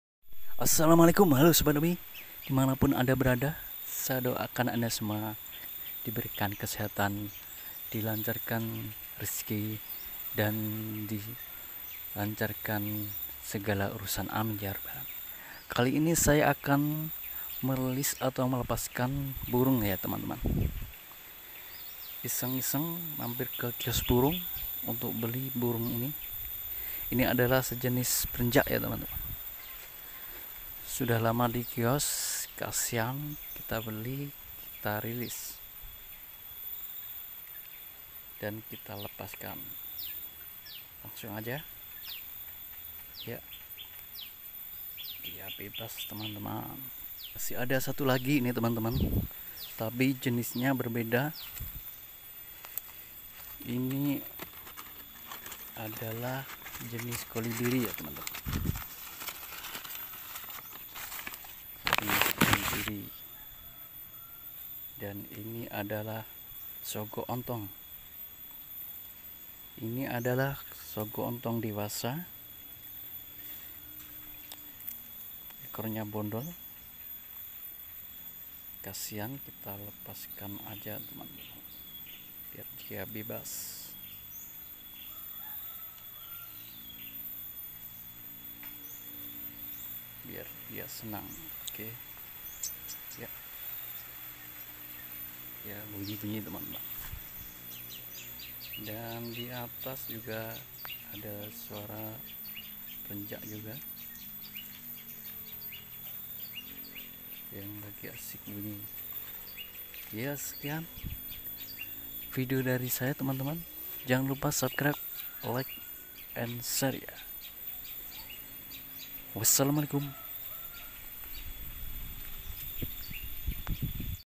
melepas burung kecil dari kios burung
iseng iseng mamapir dikios burung ,kitabeli burung melepaskannya